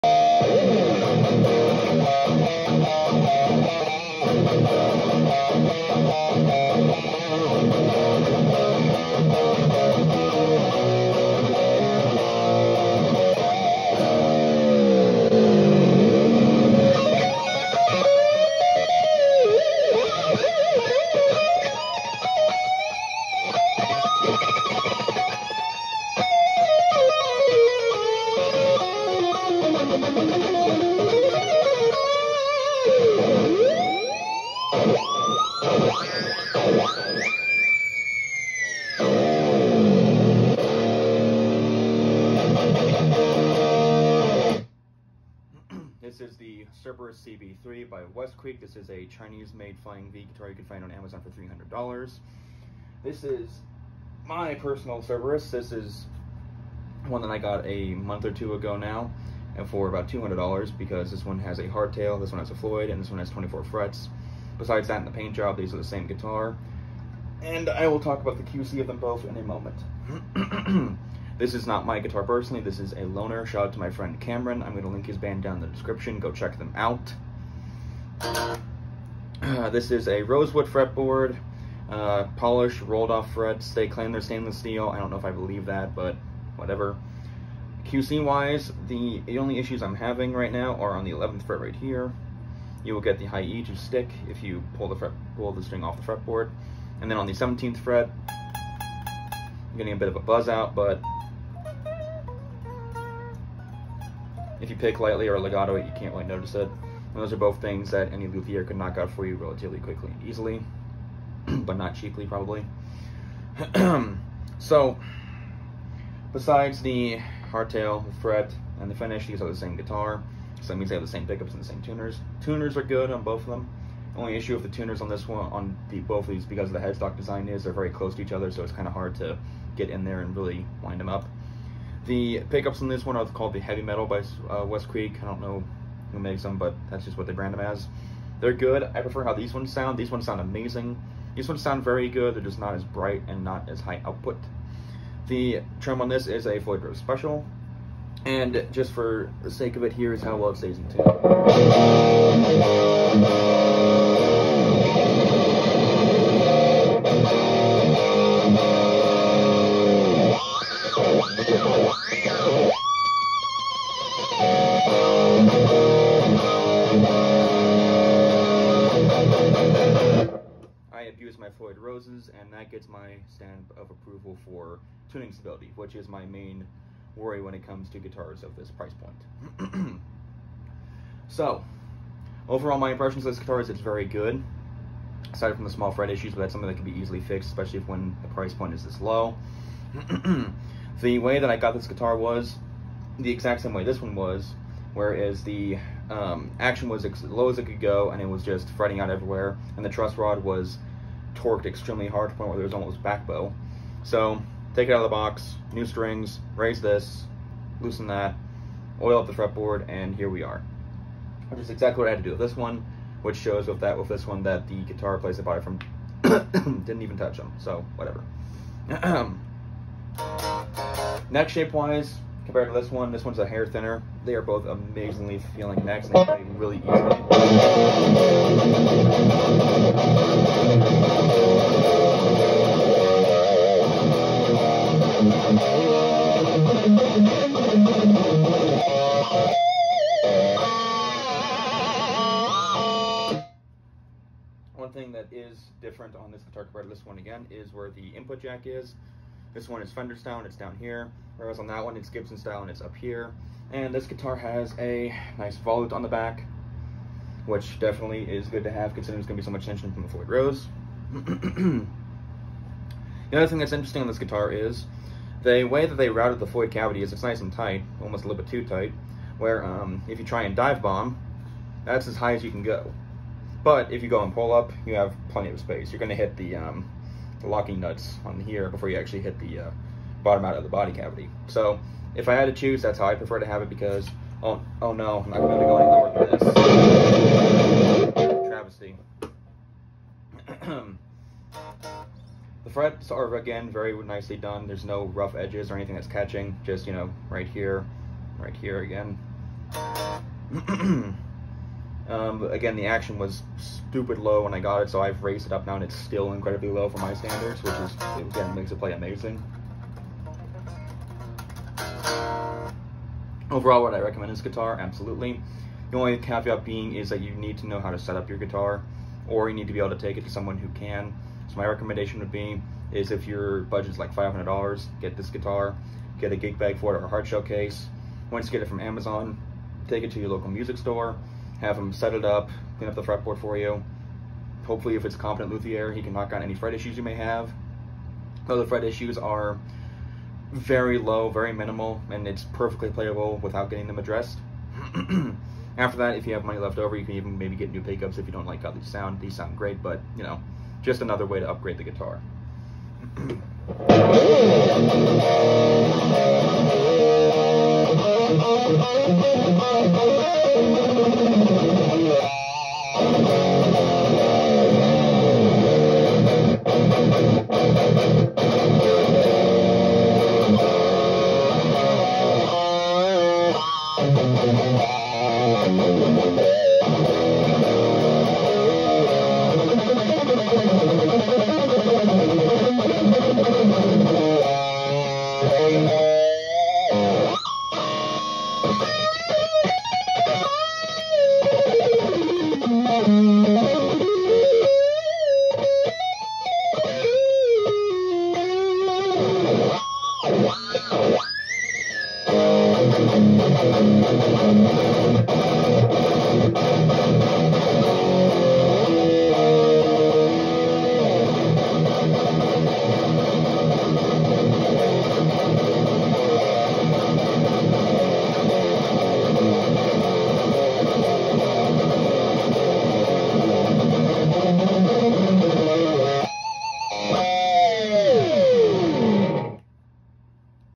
Westcreek Cerberus CB-3 Floyd Rose Review
Here’s my run through of the brand new Cerberus CB-3 by Westcreek!
What do I think of it? How’s it play? How’s it stay in tune? Watch to find out!